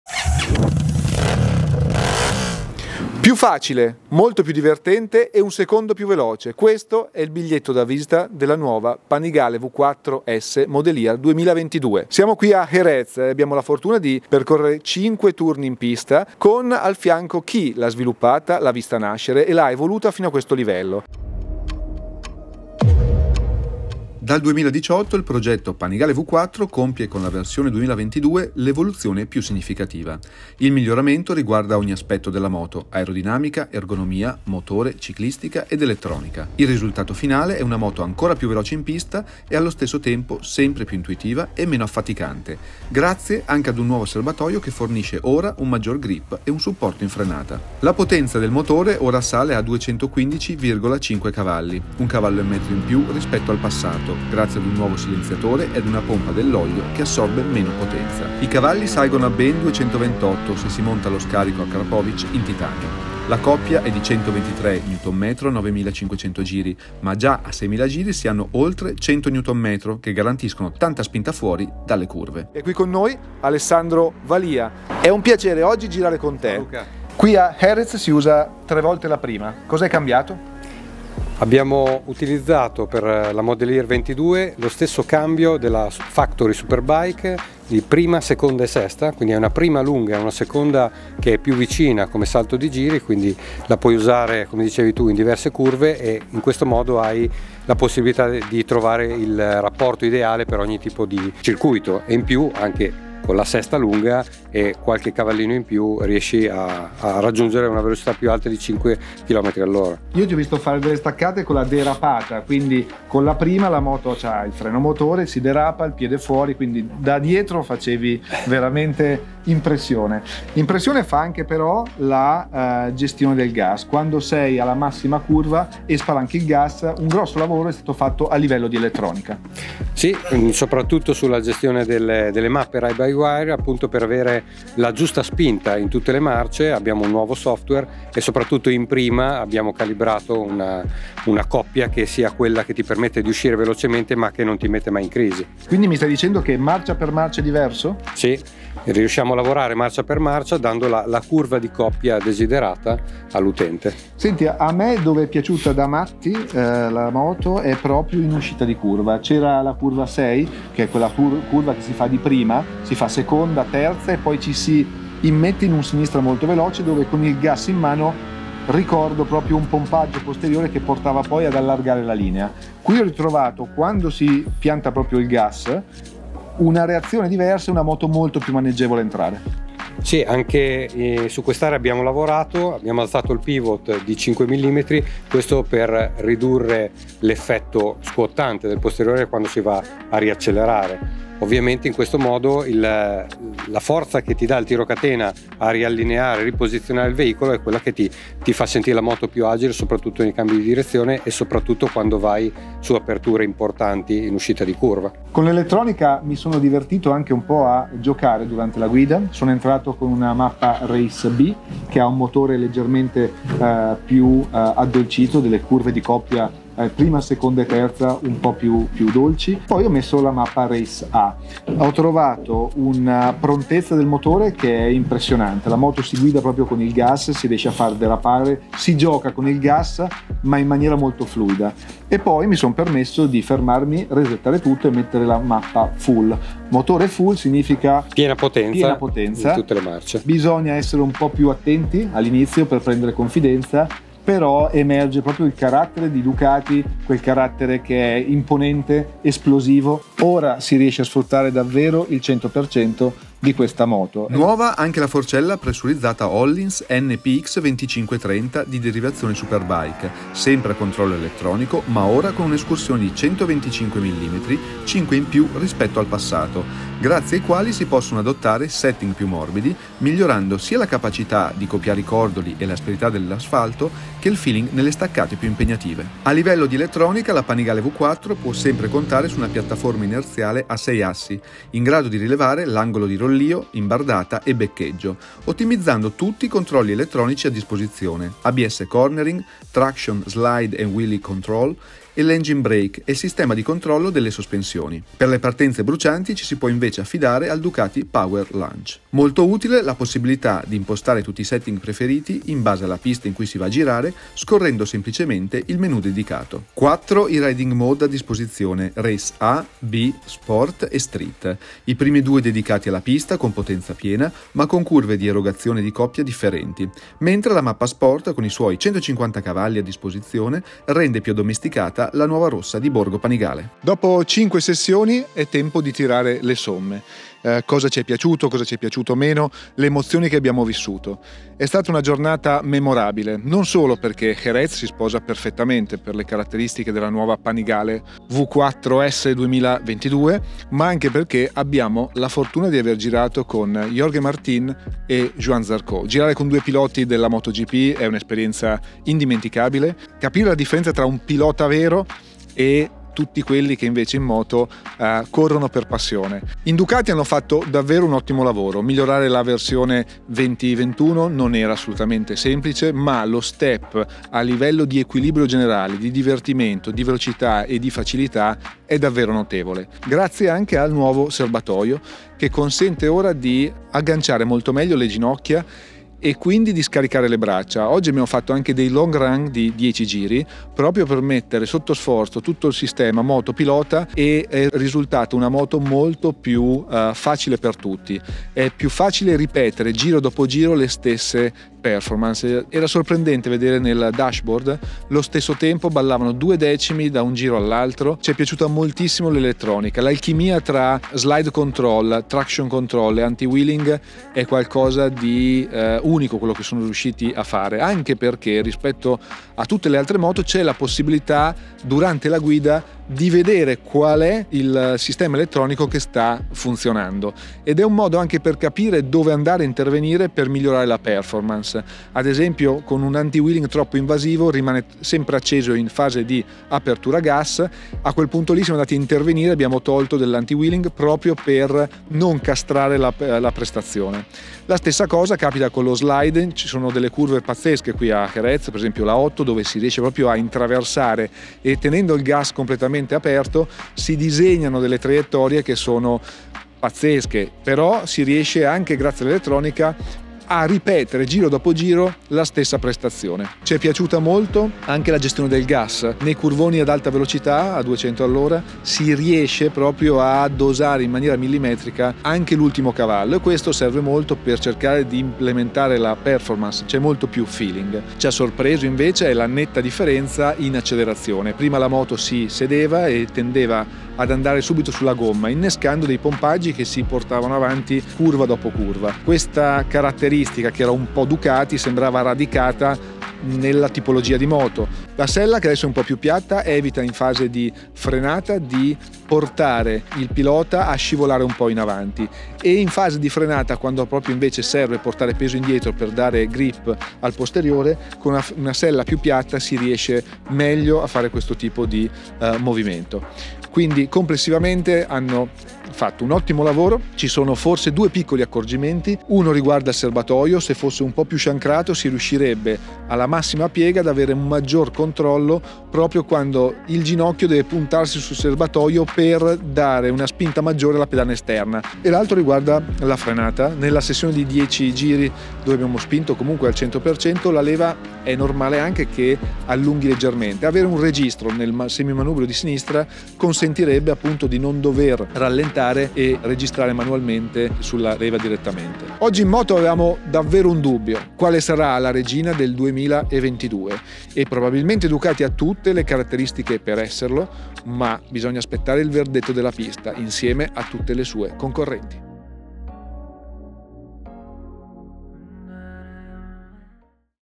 Ducati Panigale V4S 2022 | Come lei NESSUNA MAI! La prova a Jerez
Abbiamo avuto la possibilità di toccare con mano l'evoluzione della DucatiPanigaleV4S 2022, la migliore supersportiva della storia del marchio bolognese per efficacia, facilità, tecnologia e altro ancora. Ecco la nostra prova sul circuito di Jerez de la Frontera.

00:00 Introduzione
01:13 Alessandro Valia, tester Ducati entra nel dettaglio dello sviluppo
01:30 il cambio ravvicinato Superbike
02:10 gestione delle mappe e nuovo software marcia per marcia
03:00 perché è stato alzato il pivot di 5mm (anti squat)
03:30 le mappe
04:00 la mappa ‘Full’
04:15 forcella e sospensioni Ohlins elettroniche escursione 125 mm (+5 mm)
04:38 la piattaforma inerziale a 6 assi
05:10 I riding mode
05:28 le conclusioni
06:10 focus sulla nuova ergonomia serbatoio-sella
06:49 i trucchi per ottimizzare le prestazioni grazie alla nuova dashboard
09:28 cosa ci piacerebbe fosse diverso